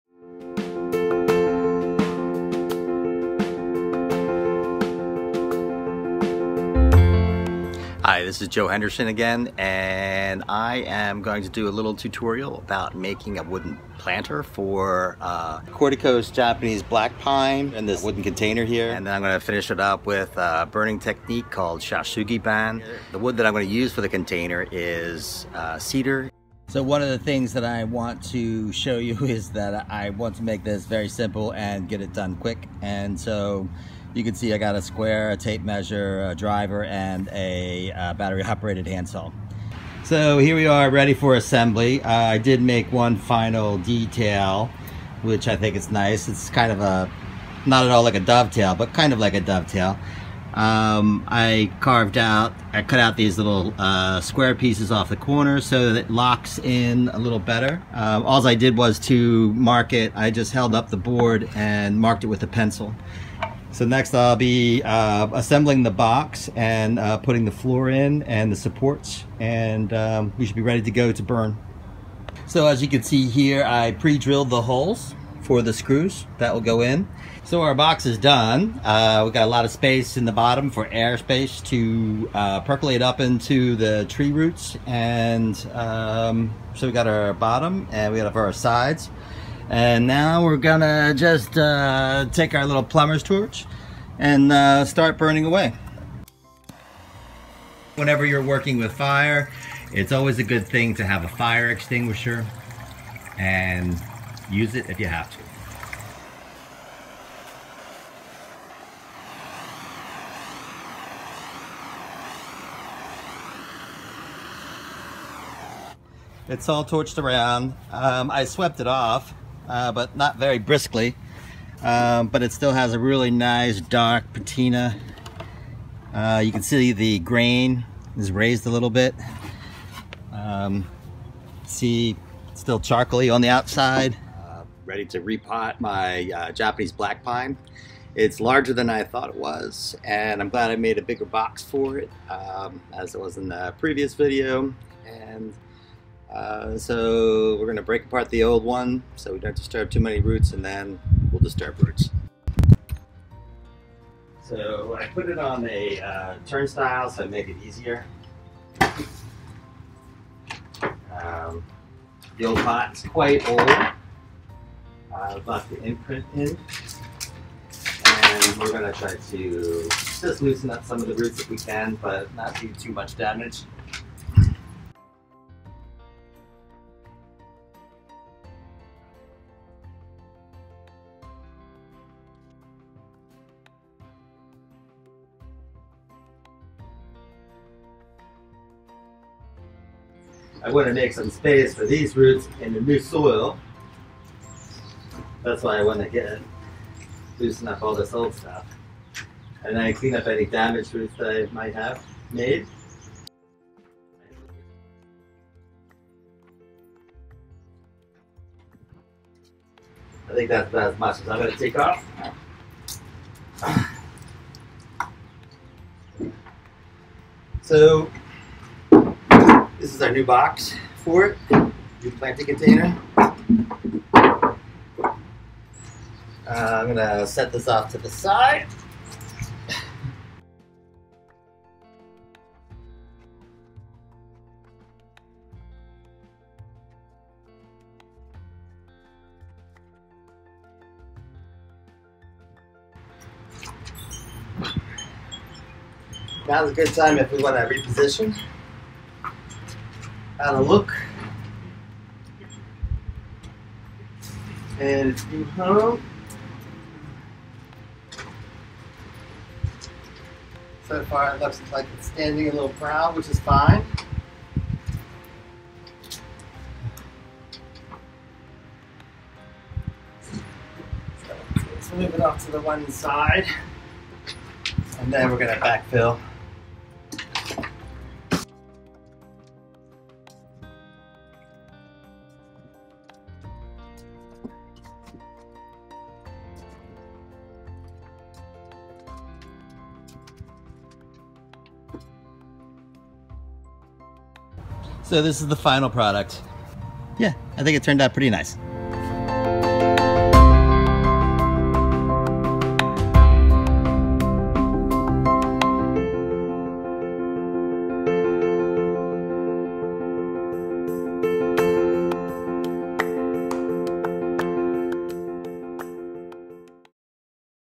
A New Container for an old Black Pine
Watch Joe Henderson as he creates a new cedar container for his old Black Pine.